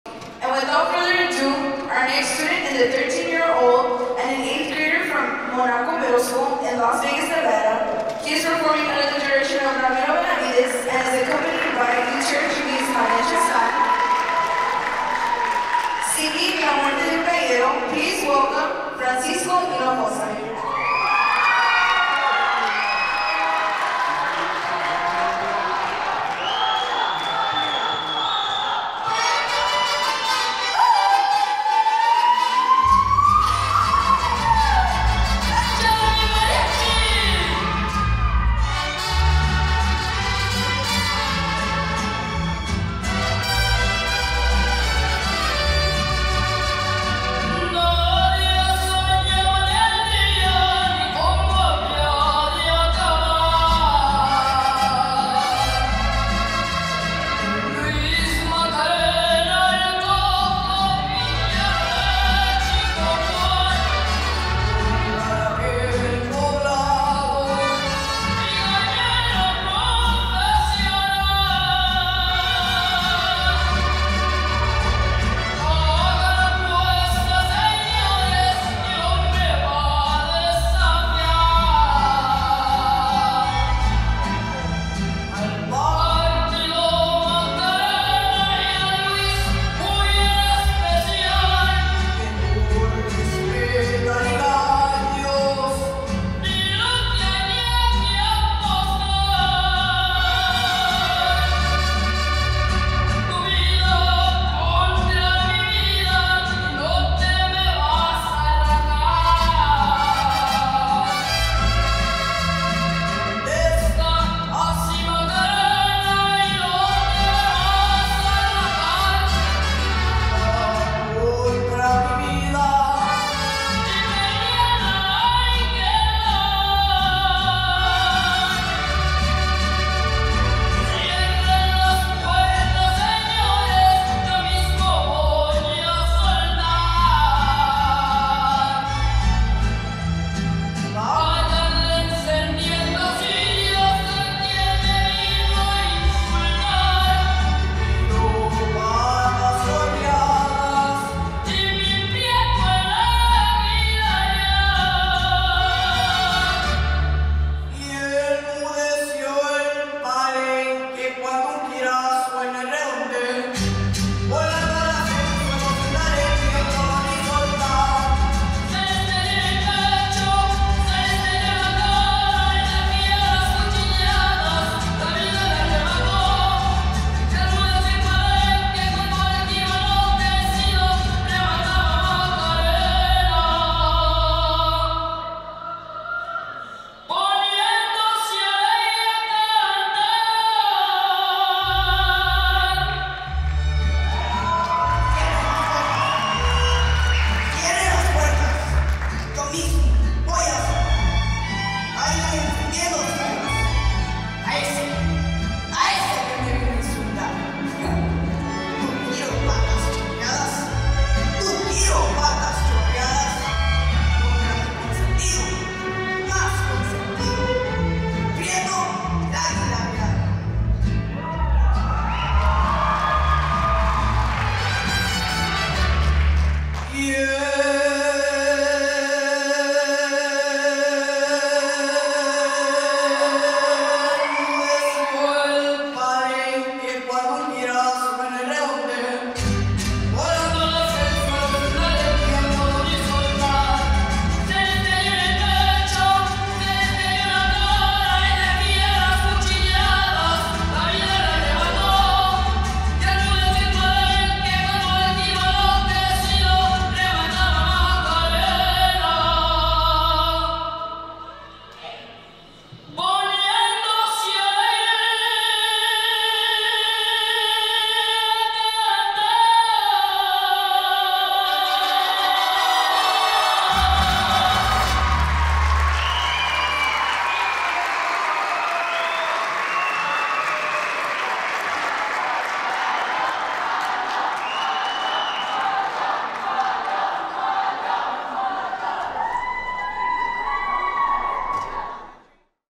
4K Video - Francisco Iniguez Hinojosa - 2022 Mariachi Extravaganza National Vocal Competition
November 18, 2022 - Francisco Iniguez Hinojosa from Monaco Middle School in Las Vegas, NV competing in the Middle School Category at the 2022 Mariachi Extravaganza National Vocal Competition.